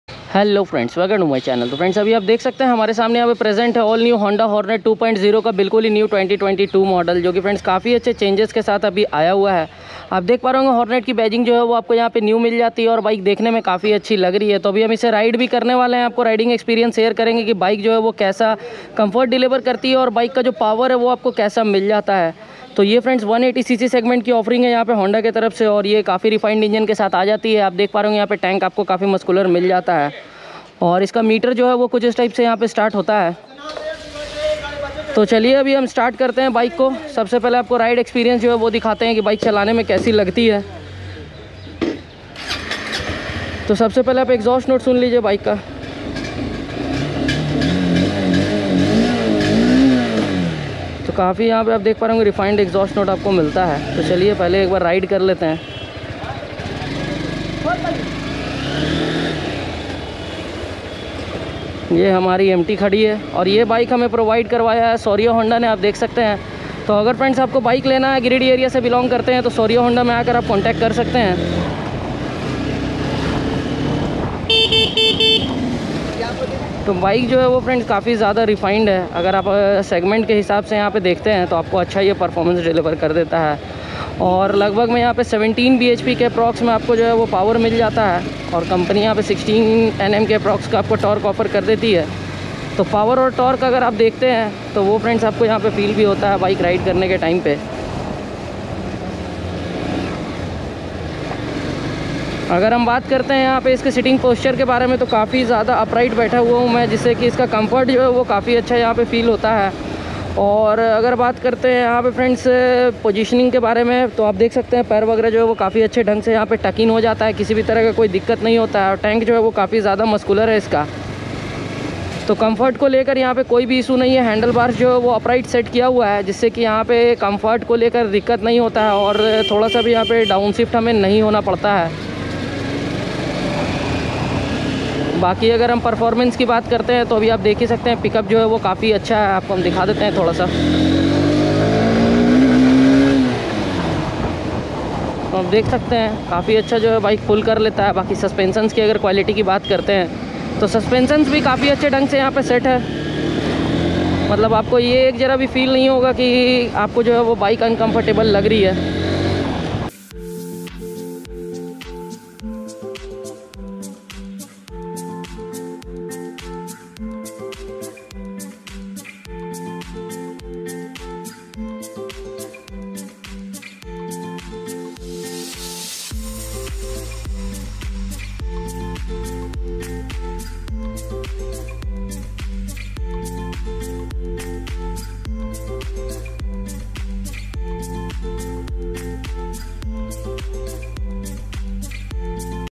All New Honda Hornet 2.0 Ka 2022 Model Is Here With New Features,New Looks & Price ~ Exhaust Sound
Hello friends,

In this video I am going to tell you about All new   honda hornet 2.0 bs6 2022 model what's new in this model...??new features?? new price??launch date??